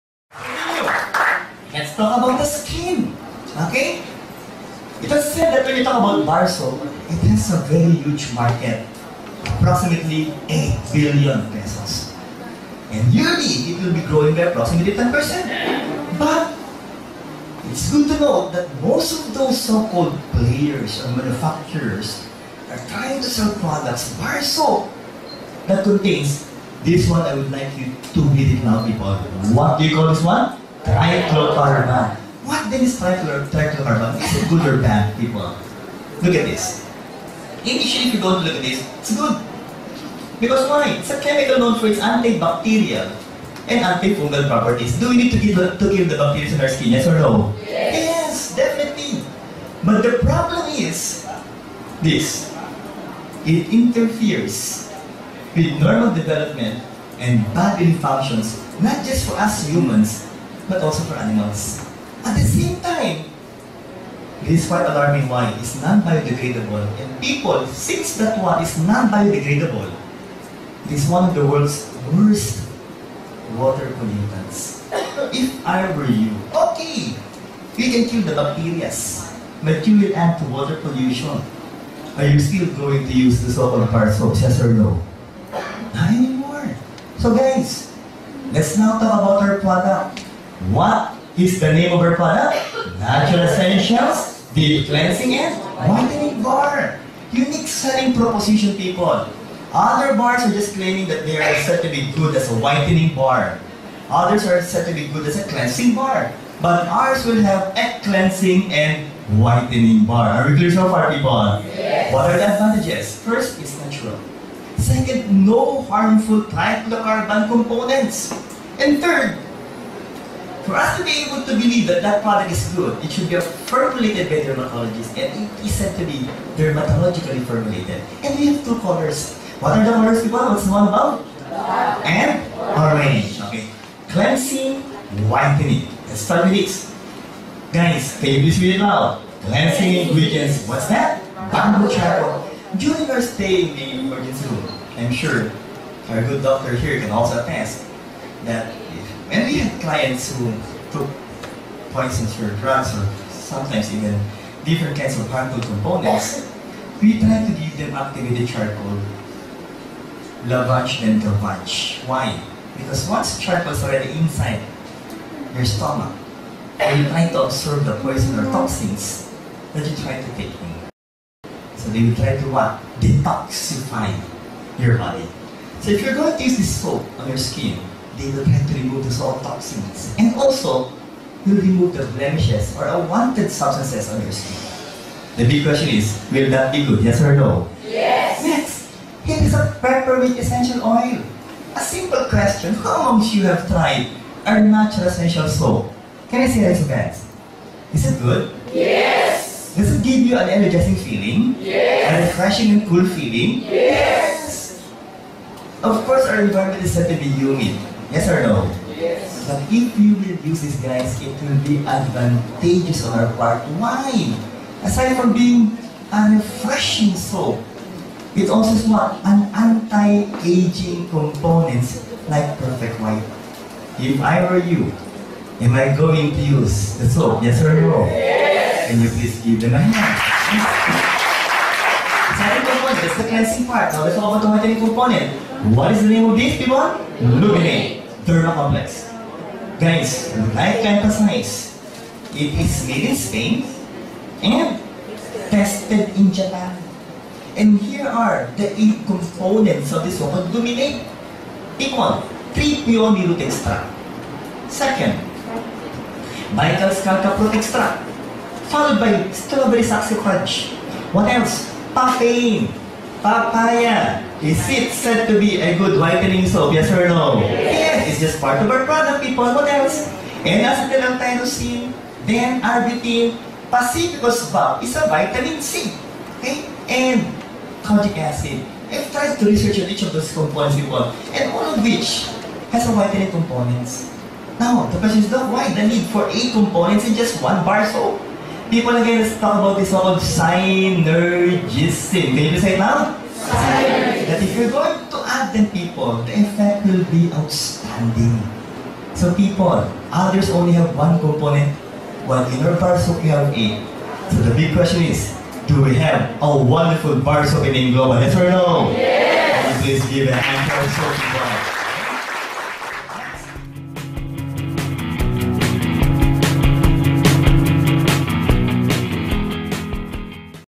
aim global naturacential soap
The most controversial soap ever made

 After a week of using this soap. . .see the difference and effectiveness. .
+ Hanap mo ba ay kojic soap?
+ Papaya soap?
+ Charcoal soap?
+ Arbutin soap?
+ Antibacterial soap?
+ Anti-aging soap?
+ Whitening soap?
Get a cleaner,smother fairer,and younger skin with every wash,Naturacentials,deep cleansing,and whitening,bar soap is 100percent natural and dermatologically formulated with no harmful chemicals.It contains Bamboo charcoal for detoxification and removal of blemishes.It also has peppermint Essential Oil for energizing, refreshing,and anti- aging.It also contains Lumin,8Derma complex which is made in Spain and tested in Japan for whitening.